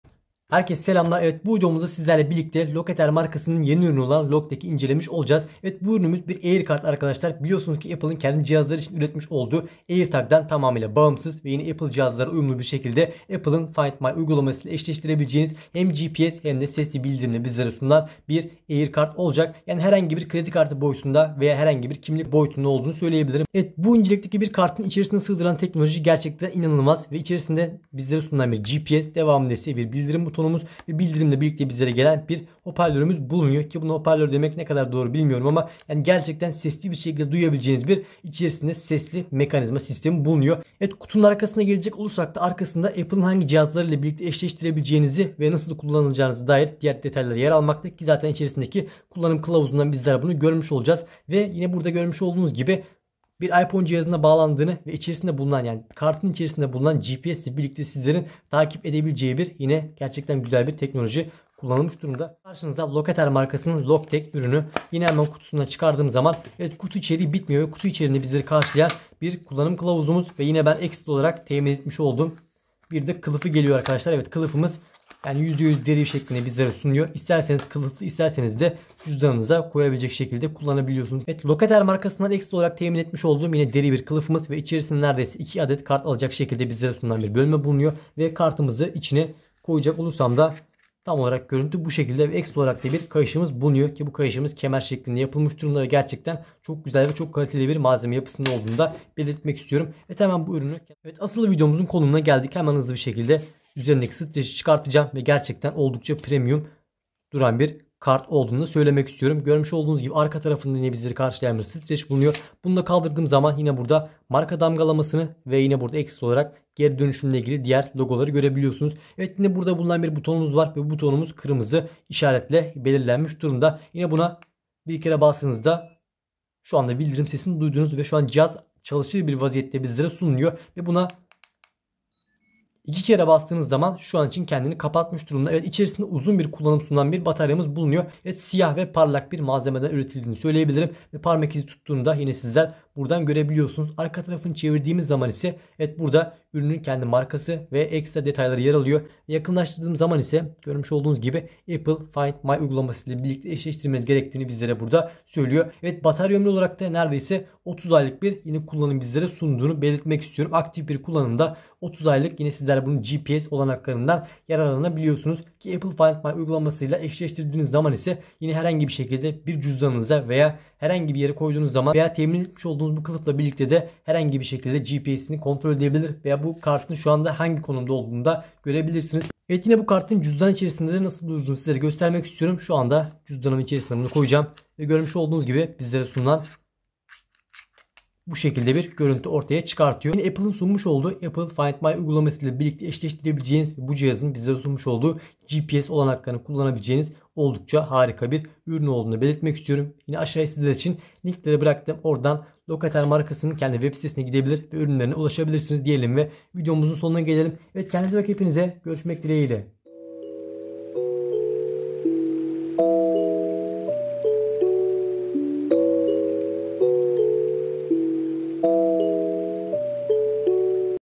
YENİ NESİL AIRCARD İLE TAKİP ELİNİZİN ALTINDA! Locater Loctag Detaylı İnceleme
Herkese selamlar,  bu videomuzda sizlerle birlikte Yeni Nesil Aircard teknolijisini inceliyoruz. Bir kimlik boyutunda olan ve barındığı teknoloji ile (GPS) ve ( Bluetooth ) paylaşımını bizlere sunan bu Aircard, Apple Find my uygulaması ile senkronize edildiğinde, bizlere konum ve yer takibi özelliği sunuyor. Sesli bildirim ve uzun batarya kullanımı ile oldukca başarılı...  daha fazlası videoda sizlerle. iyi seyirler

🌐Greetings everyone, in this video we are examining the New Generation Aircard technology with you. This Aircard, which is the size of an ID and offers us (GPS) and (Bluetooth) sharing with the technology it hosts, offers us location and location tracking when synchronized with the Apple Find my application. It is very successful with voice notification and long battery usage... more with you in the video. good watching